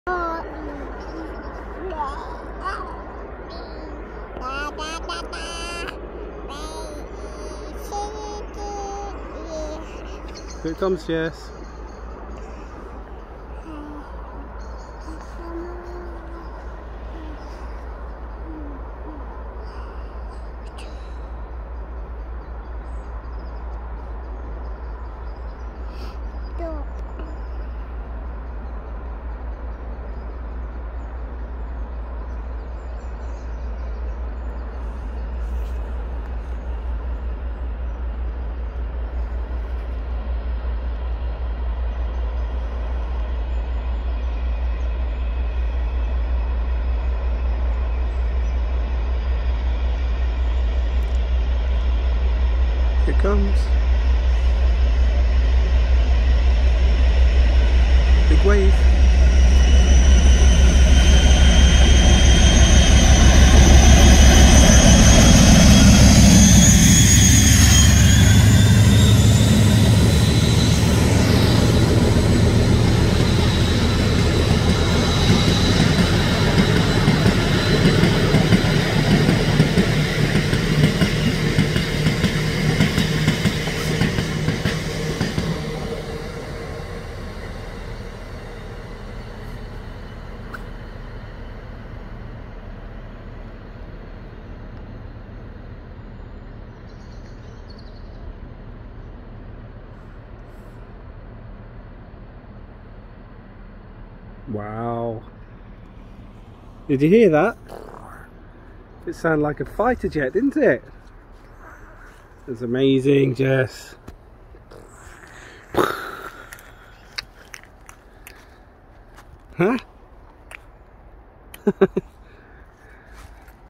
Jessica sees a class 56 or was it a fighter jet?
Simply an incredible sounding locomotive!

Train 612Z 1011 Tyne S.S. to Pinnox Branch Esso Sidings